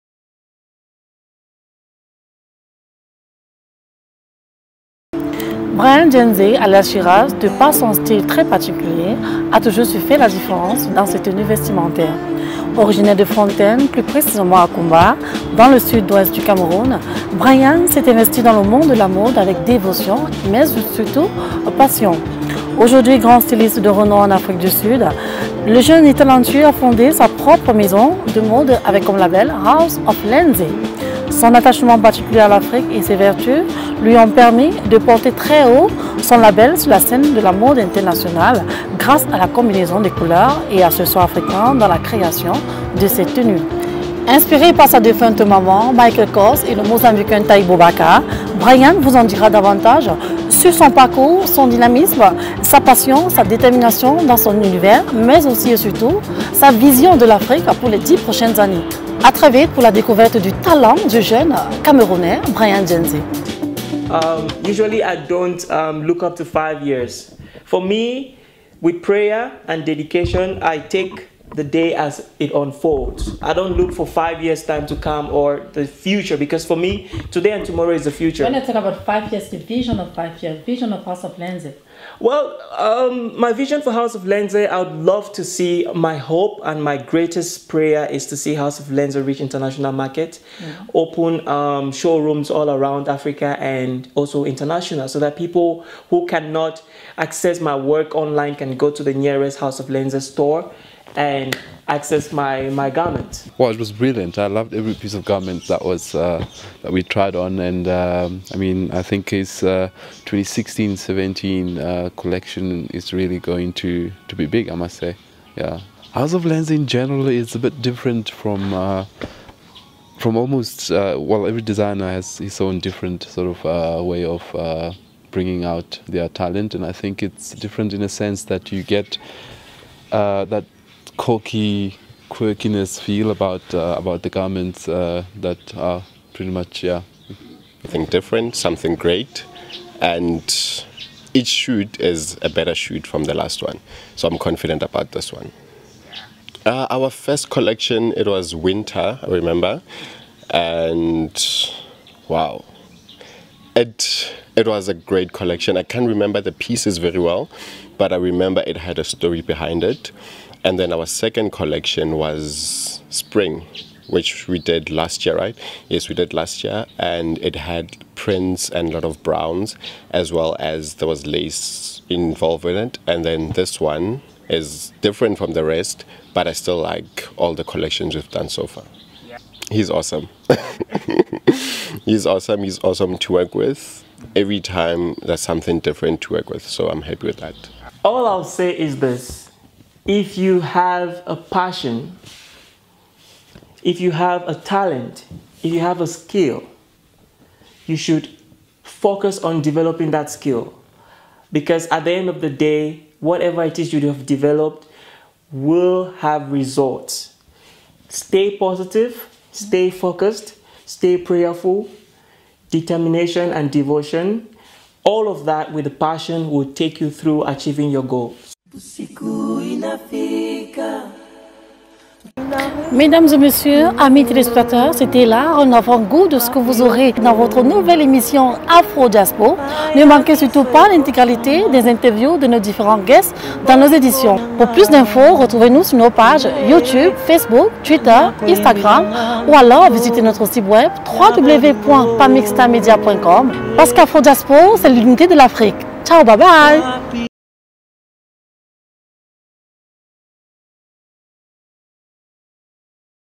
Afro Diaspo Show
Discover Brian Ndienze,

A Cameroonian fashion designer with incredible talent.